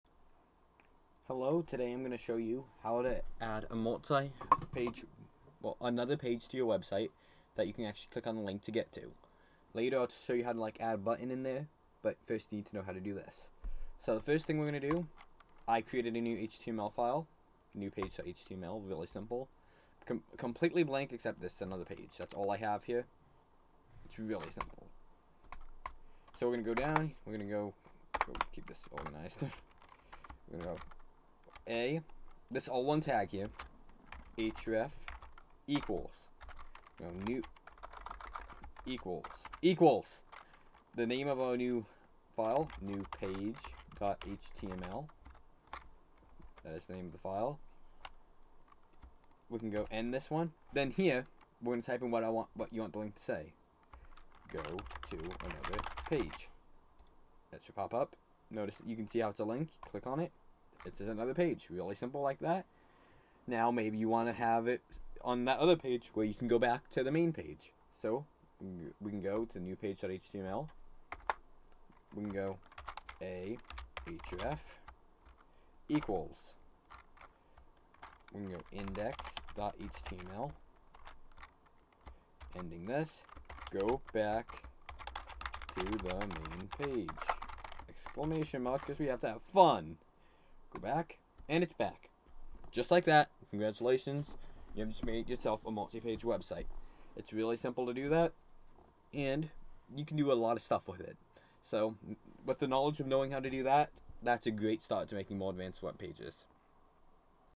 Web Development: Multiple Pages in HTML (7)
This is part 7 of my Web Development tutorial series.

VIDEO CONTENTS:
How to set up multiple pages in 1 website using HTML